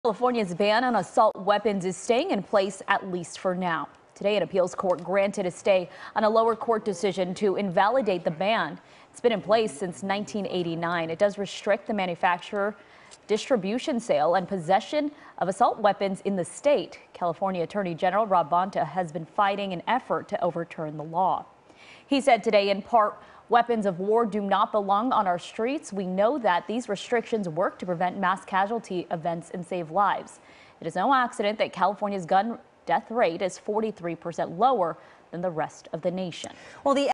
Appeals court overturns decision to invalidate assault weapons ban in California
An appeals court has granted a stay on a lower court decision to invalidate California's ban on assault weapons, which has been in place since 1989.